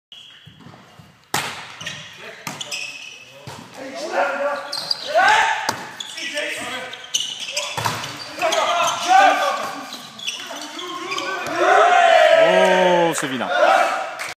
Corvi1 / Solaize 2014 - Courte d'Edwin contrée et passe longue de Damien
Match CORVI1 / Solaize du 10 février 2014
3ème set
Edwin se fait contrer en courte mais parvient à récupérer le ballon.
C'est finalement Damien qui pousse la balle sur la bande du filet et qui marque le point.